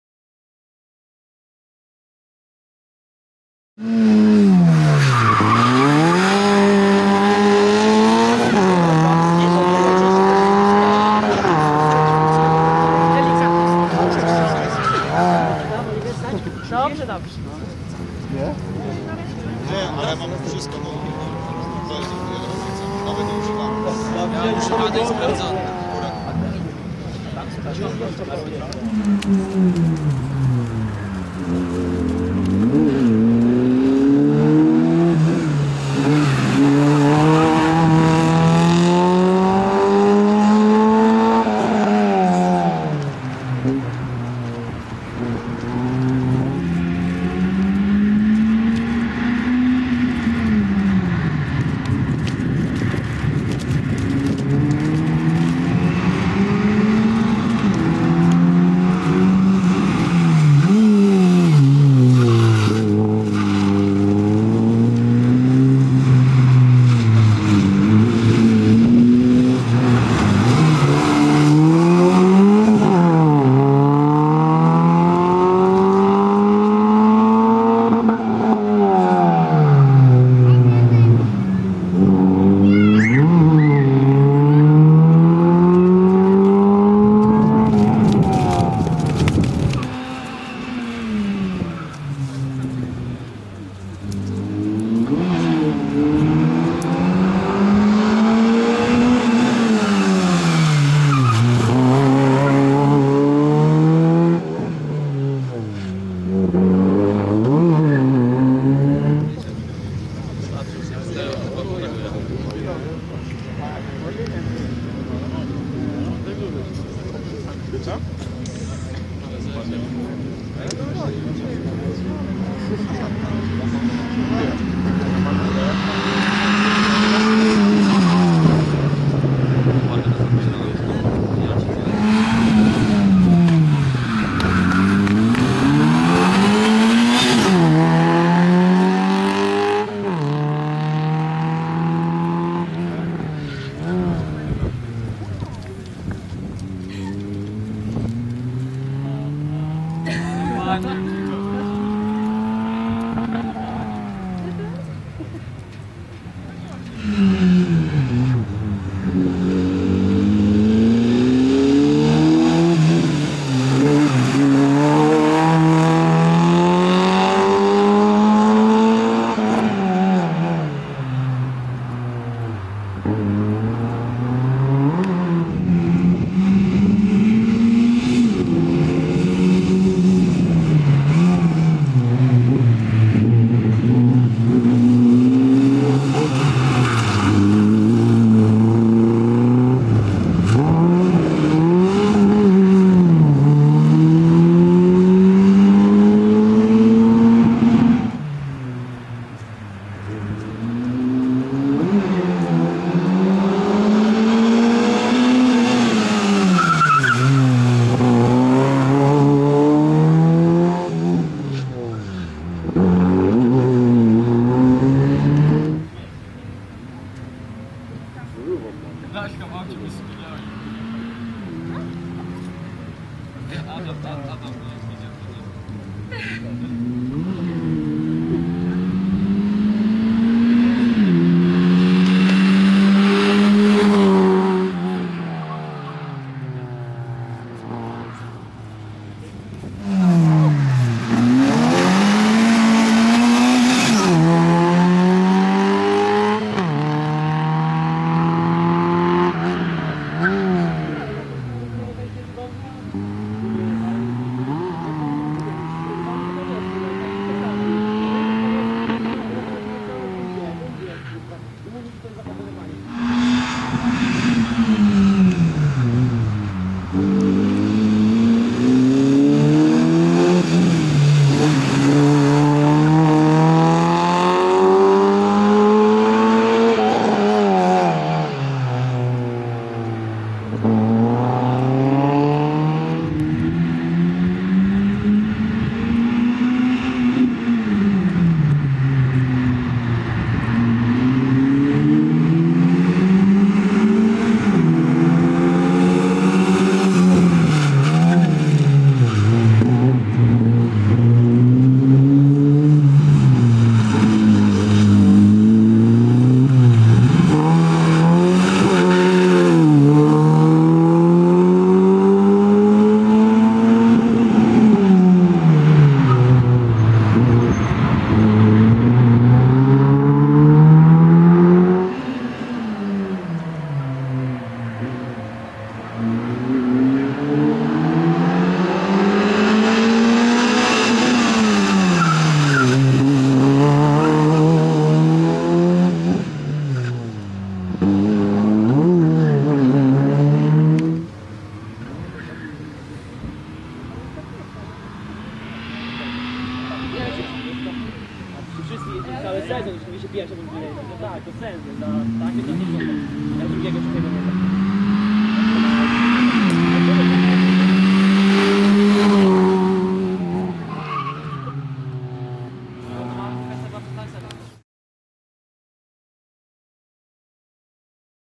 Zięba Dariusz / Zięba Dawid - Fiat SC - KJS Babski Super OeS Gorlice 06-03-2016
Zięba Dariusz / Zięba Dawid
Fiat SC
KJS Babski Super OeS LASCAR
1 runda Mistrzostw Powiatu Gorlickiego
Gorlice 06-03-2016